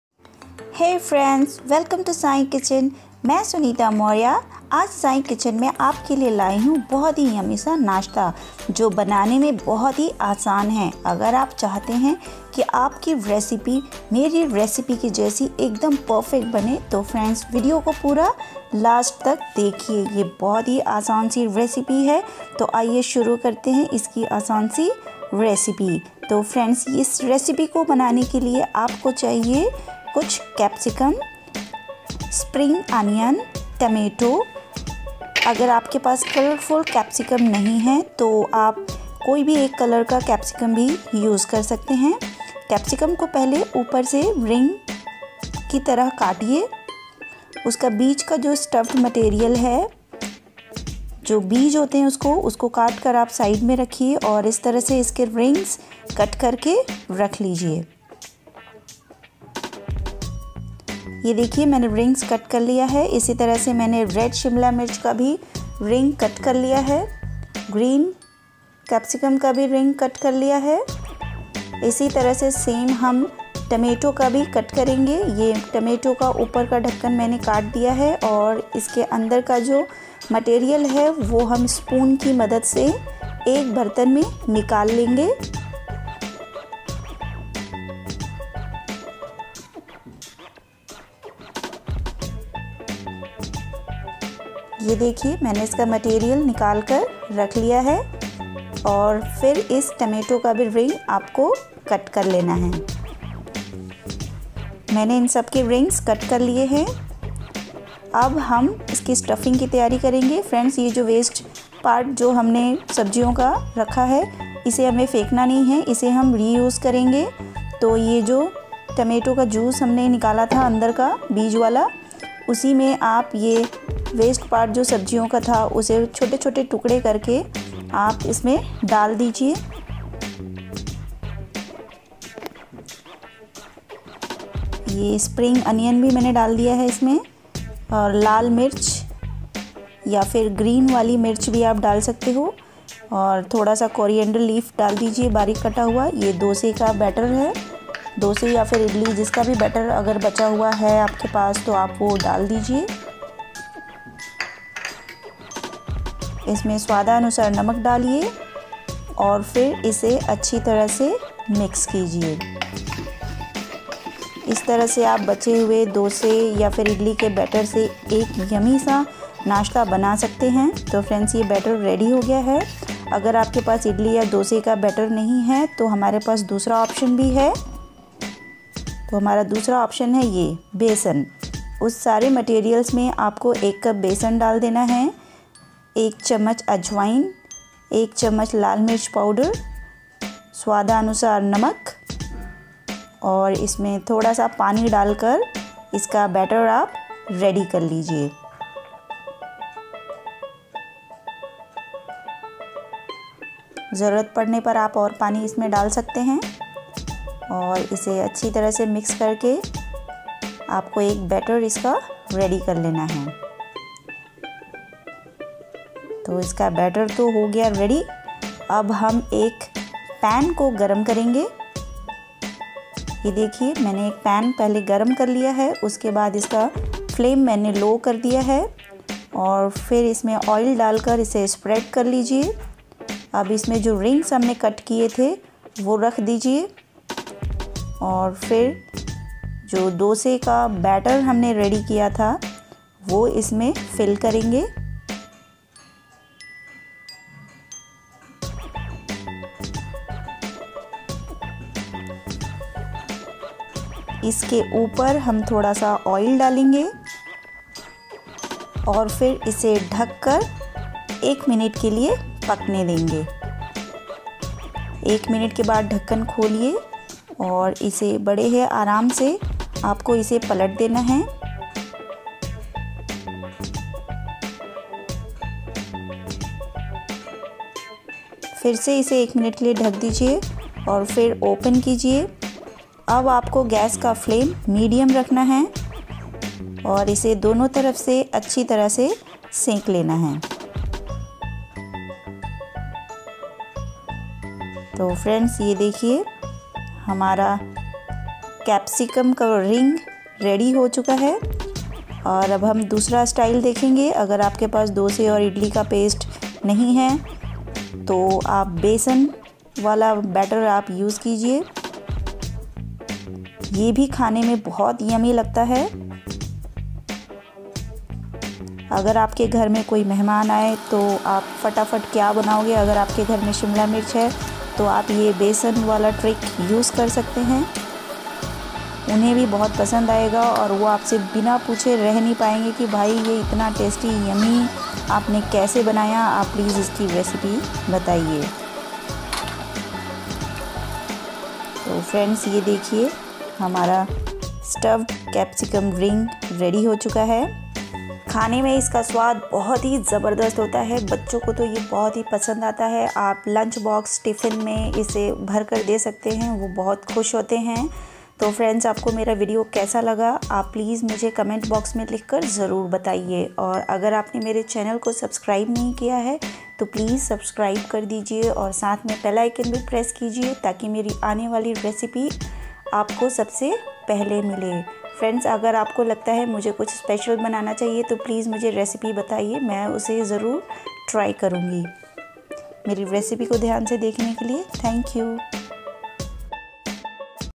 शिमला मिर्च का ऐसा टेस्टी नाश्ता आपने कभी नही खाया होगा | Stuffed Capsicum Rings| Bell Pepper recipe
Stuffed Capsicum Rings || Bell Pepper Rings|| Easy evening snack || Starter

These bell pepper rings are quick and easy to make and a real delight. Make these stuffed capsicum for breakfast, snack, serve it as an appetizer or may be as a side for lunch or dinner.